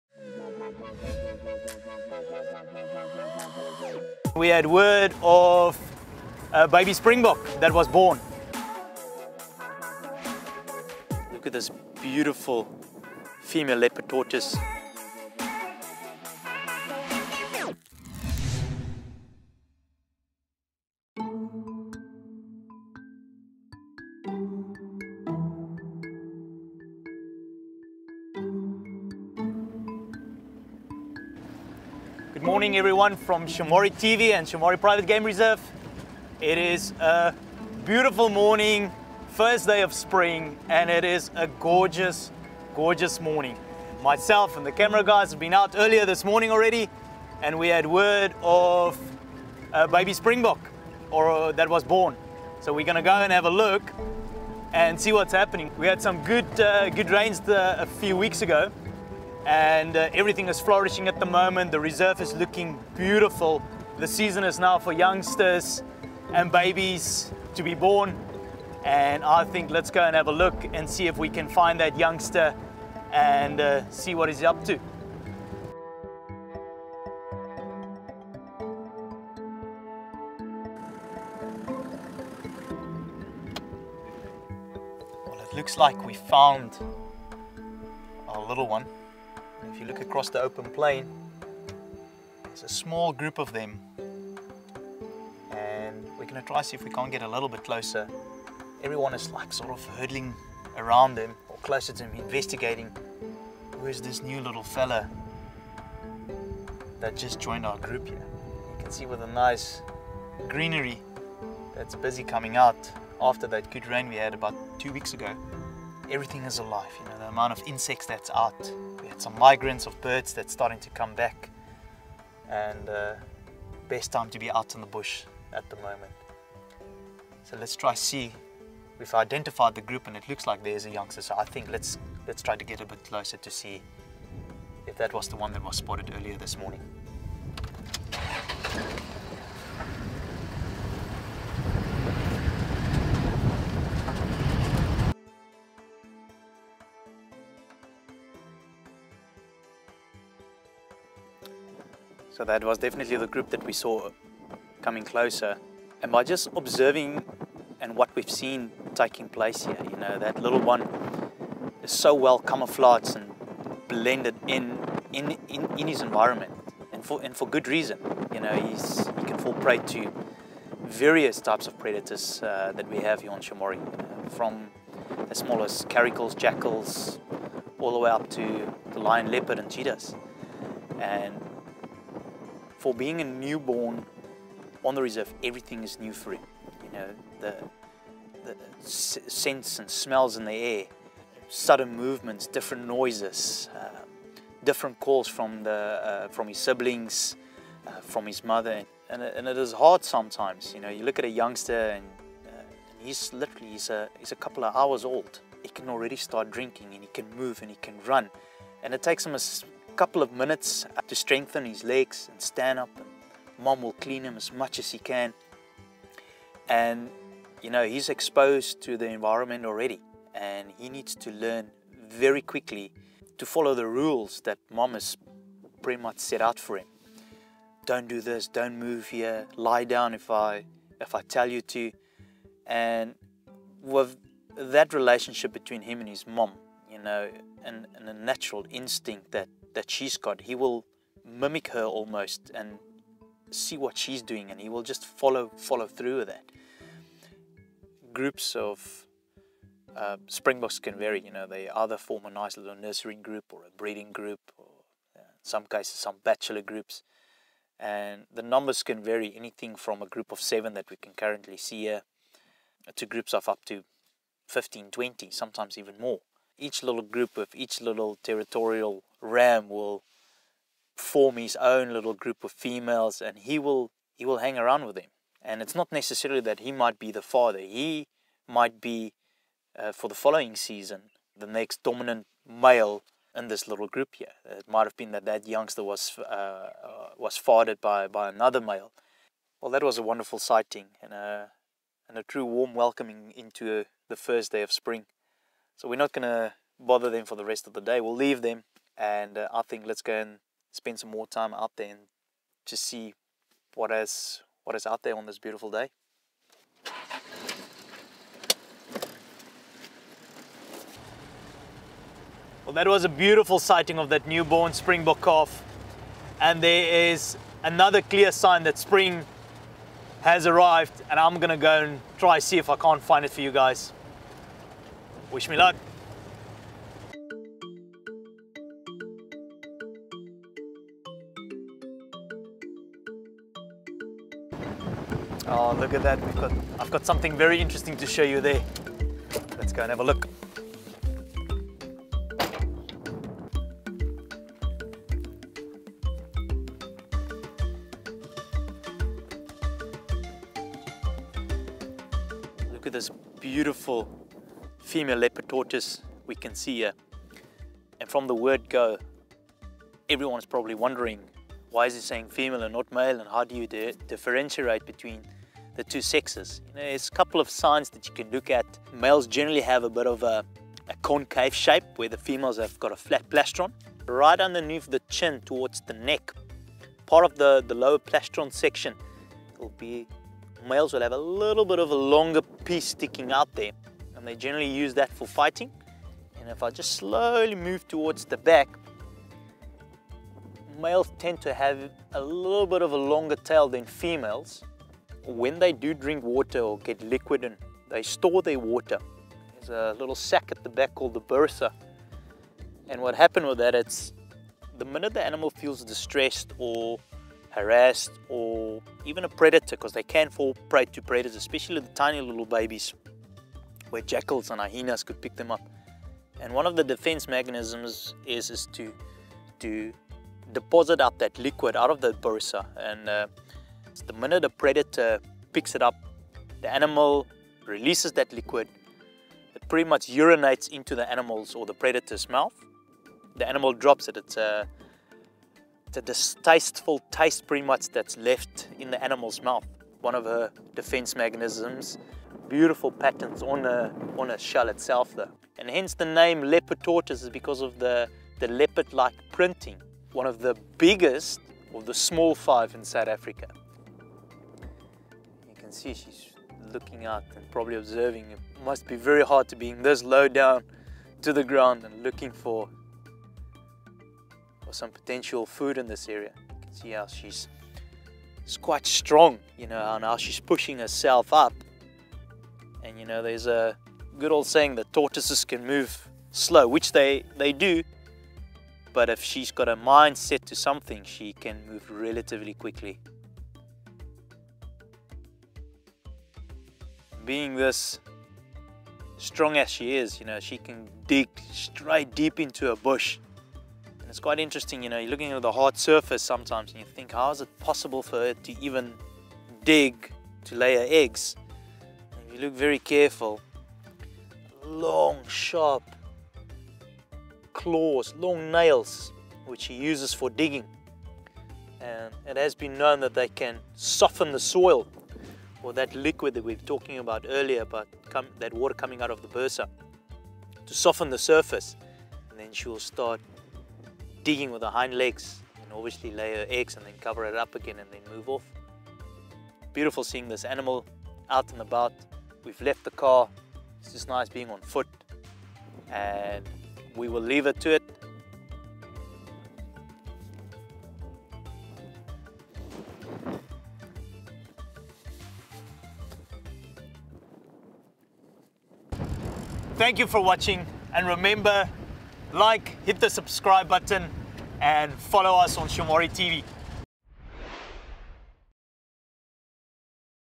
Spring Arrives: A Baby Springbok takes its first steps, and up close with a Leopard Tortoise
Spring is an exciting time to be in the bush. Many animals give birth shortly after the start of spring, as it provides the best weather conditions to successfully rear their young. With less chance of harsh weather, rising average temperatures and the longer days, many animals have a better chance in finding nourishment for themselves and their young. With the arrival of Spring, ranger Jan Dykema, heads out on Shamwari Private Game Reserve to see what he can find on game drive.